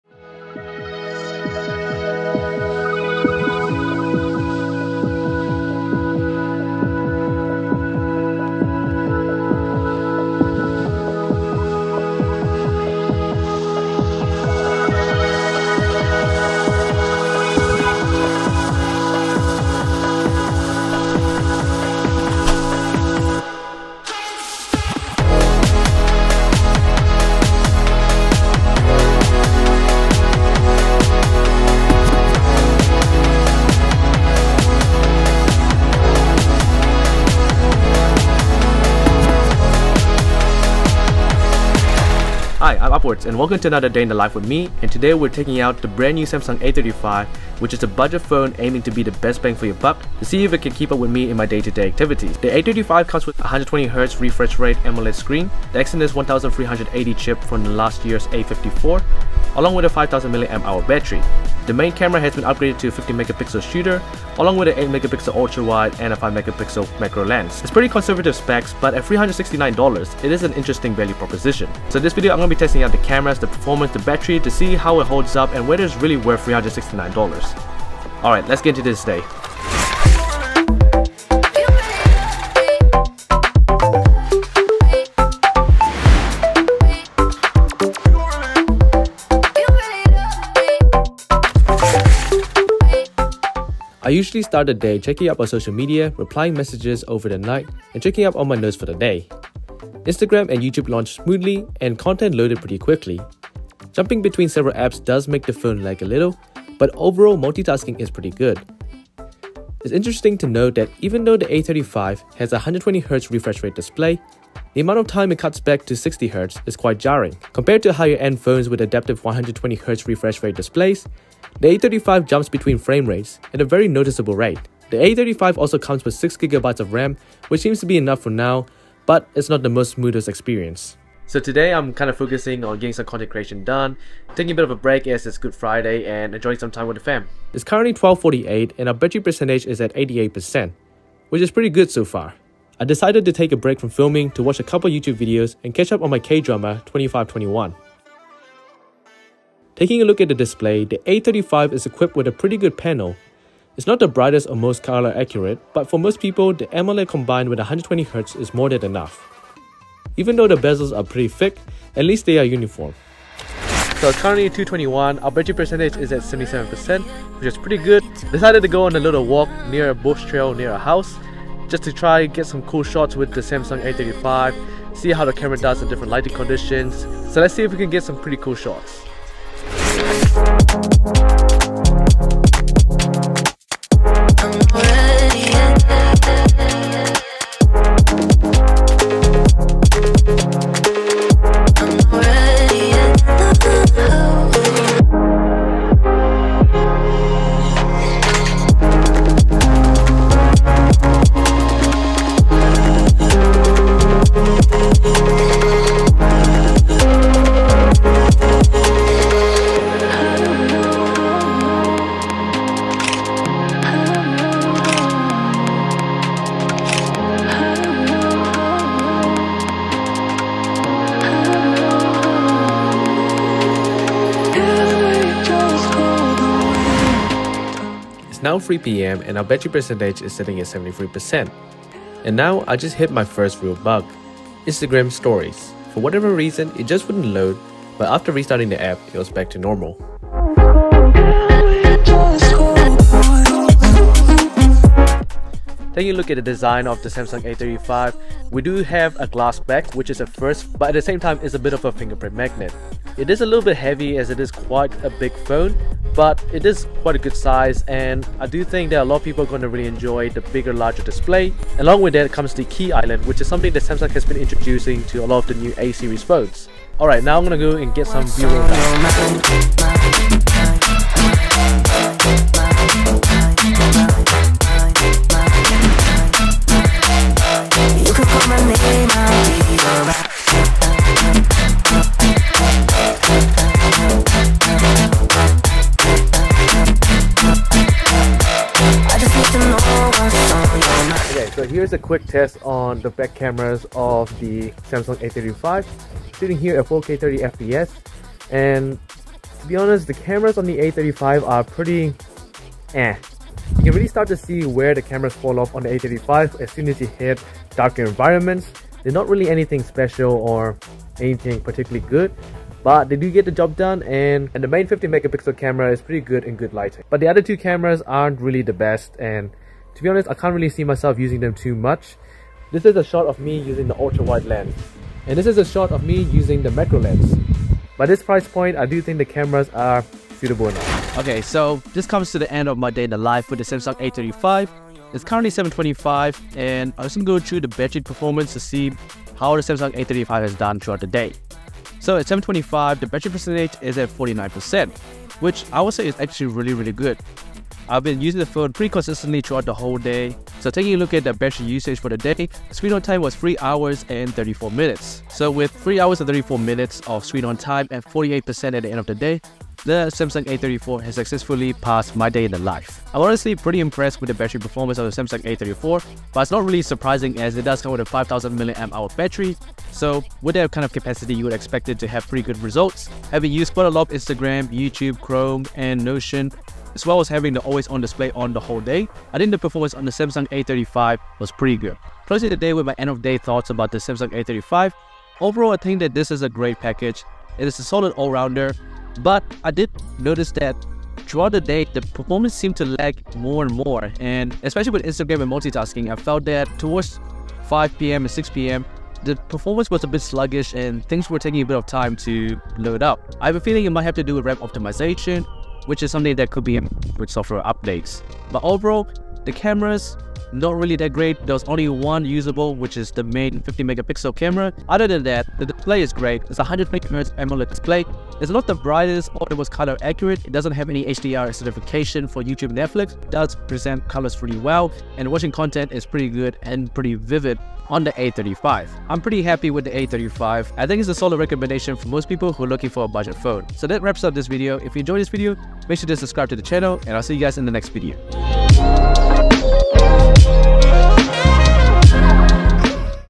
Samsung Galaxy A35! - A Real Day In The Life
The new Samsung A35 has just been released and it's a budget phone aiming to be the best bang for your buck. So in this video, I take the Samsung A35 out on a day in my life to see how it holds up to my daily tasks and usage. Join me as I test out the performance, cameras, display and battery performance!